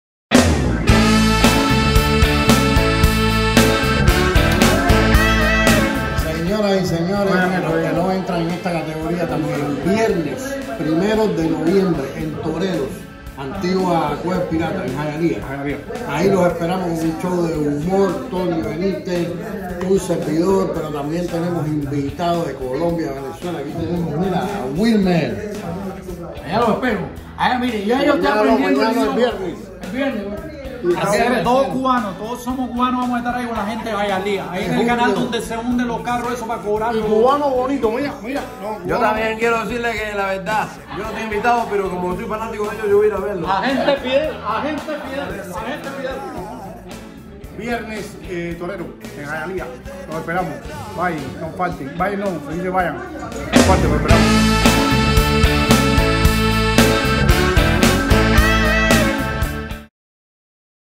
Viva la Comedia en Hialeah - Viernes 1 de Noviembre en Toreros
Ven a reirte con el mejor show de comedia de Miami!
Por primera vez en Hialeah! con los geniales comediantes cubanos Iván Camejo y Tony Benítez, además con la participación del comediante colombiano Julio Rodríguez y el anfitrión Wilmer Ramírez!
Te esperamos a pasar una noche muy divertida!